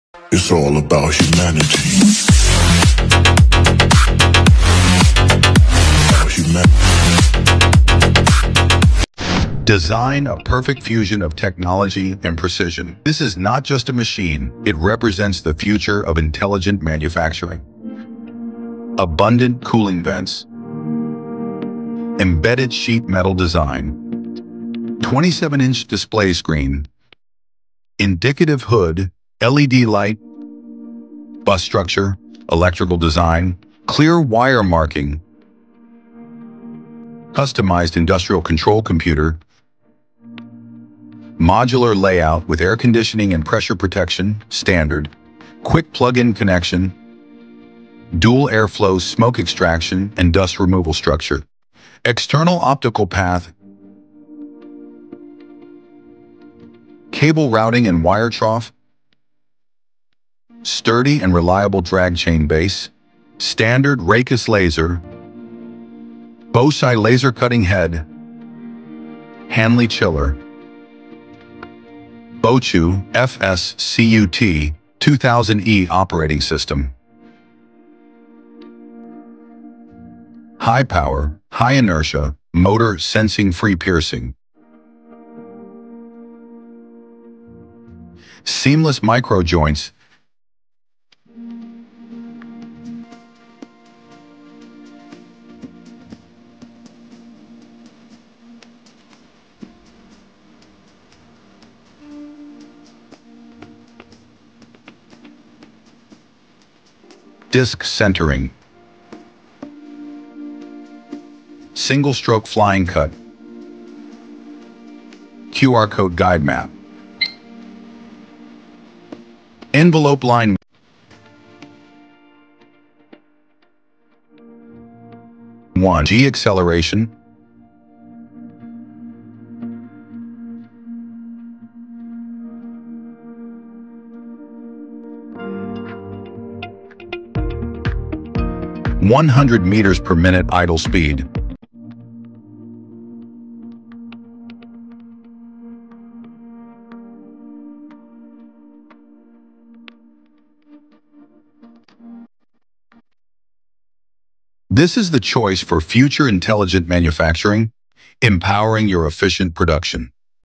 SF-Flying Series V2.0: The Fiber Laser Cutter for U.S. Manufacturers & Dealers
Introducing the SF-Flying Series V2.0 Fiber Laser Cutting Machine

The SF-Flying Series V2.0 represents the future of intelligent manufacturing. Designed with a perfect fusion of technology and precision, this machine delivers outstanding performance and advanced features.

Innovative Design Features:
Embedded sheet metal design
Abundant cooling vents
27-inch display screen
Indicative hood LED light
Bus structure electrical design
Clear wire marking
Customized industrial control computer
Modular layout with air conditioning and pressure protection (standard)
Quick plug-in connections
Dual airflow smoke extraction and dust removal structure
External optical path
Sturdy and reliable drag chain base
Performance Highlights:

Standard Raycus laser
Bochu FSCUT2000E operating system
Boci laser cutting head
Hanli chiller
High-power, high-inertia motor
Sensing-free piercing
Seamless micro-joints
Disc centering
Single-stroke flying cut
QR code guide map
Envelope line
1G acceleration, 100m/min idle speed

for exclusive fiber laser cutter builds and content!